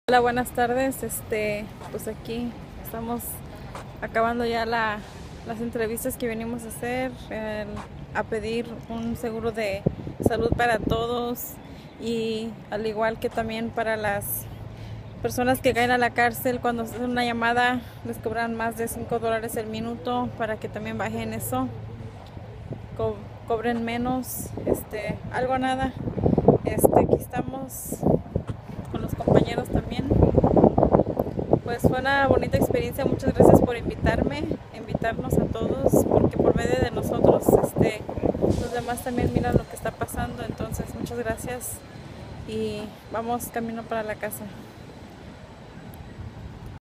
Despedida el dia en Sacramento ca.
Pidiendo un seguro de salud para todos dia del immigrante en Sacramento 5/19/14 gracias a todos por invitarnos mil gracias